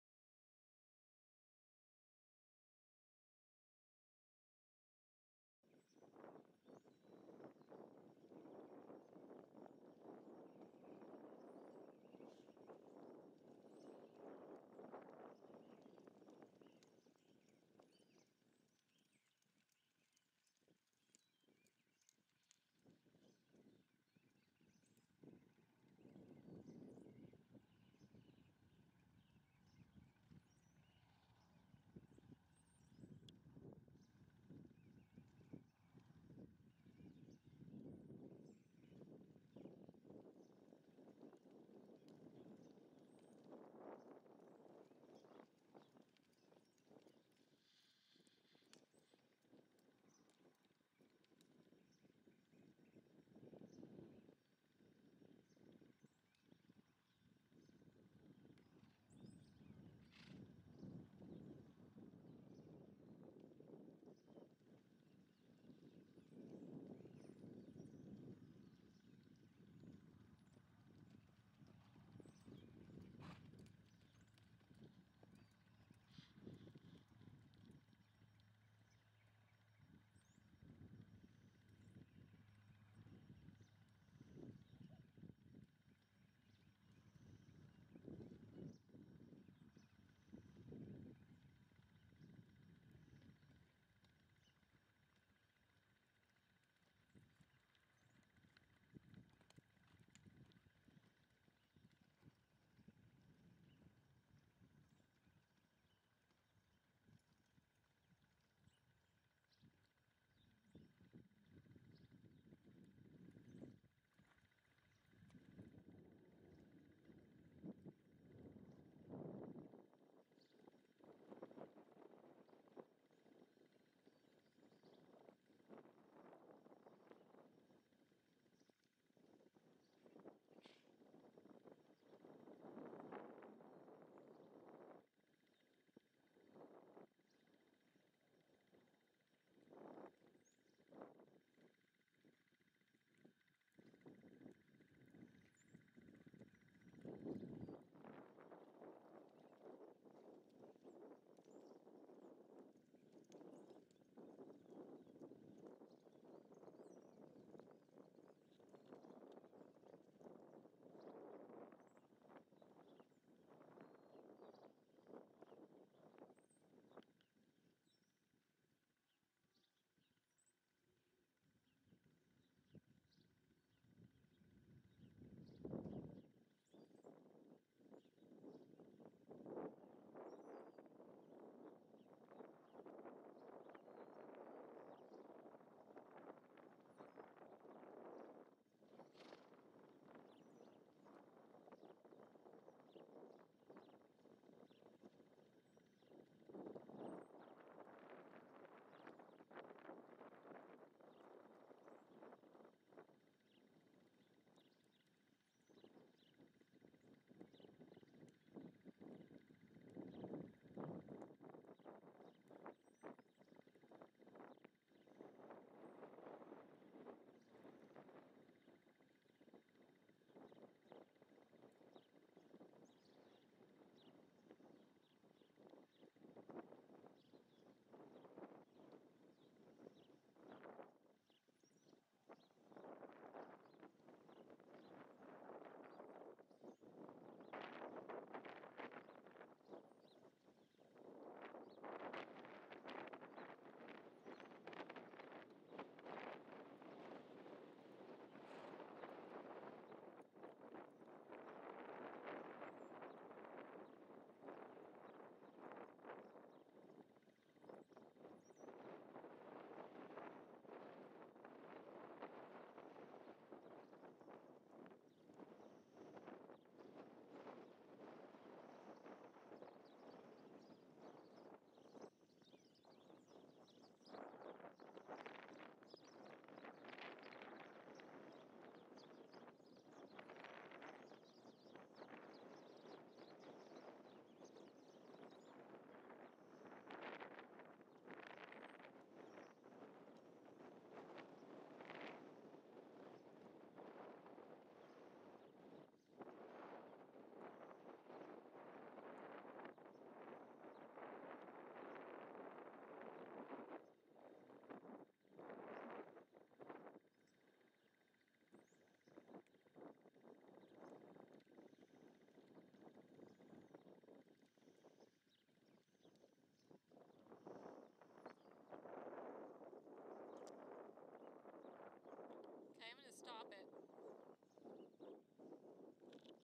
2005 APHA ALL-AROUND GELDING/LOTZA TE TIME SOLD!
HERE IS YOUR TRUE TEXAS WEEKEND WARRIOR, OVER 600 APHA PTS IN MOSTLY YOUTH, NOVICE AND AMATEUR EVENTS!!
CT CAN DO WP, HUS, SHOWMANSHIP, HORSEMANSHIP, TRAIL AND EQUITATION, AWESOME PATTERN HORSE!! CURRENTLY IN TRAINING AT BILLER PERFORMANCE HORSES, READY TO GO SHOW TODAY!!